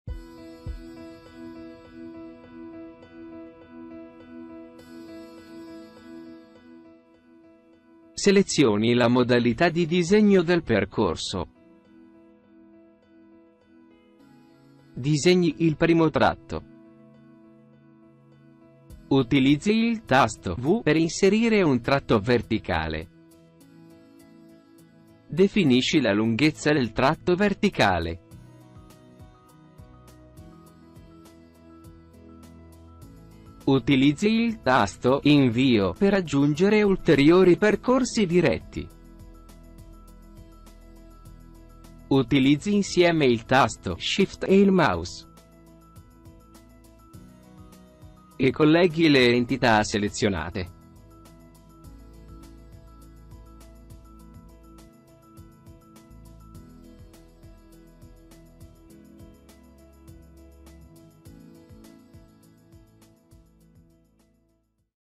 Tutorial Impiantus-ELETTRICO -Percorsi con comandi da tastiera - ACCA software
Guarda come disegnare i percorsi utilizzando i comandi da tastiera: "V", "Shift", "Invio" con Impiantus-ELETTRICO, Il software all-in-one per la progettazione di impianti elettrici in bassa tensione. Se vuoi scopri di più sul software Se vuoi, scarica il software utilizzato nel video